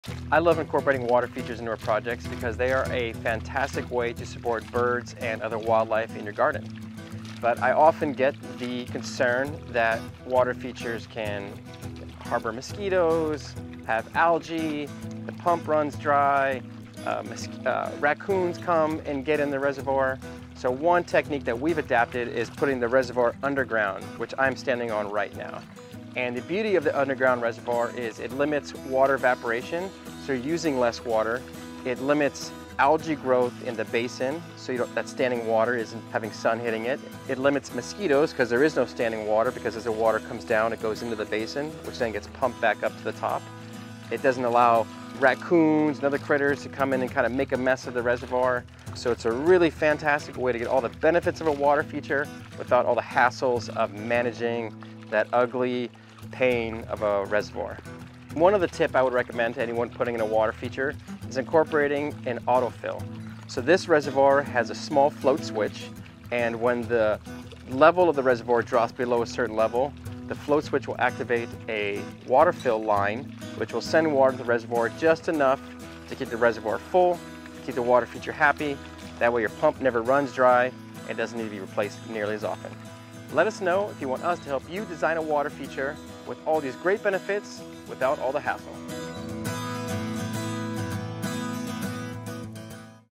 Water Feature Reservoir
Benefits of below ground reservoirs. All our water features are built using Aquascape products.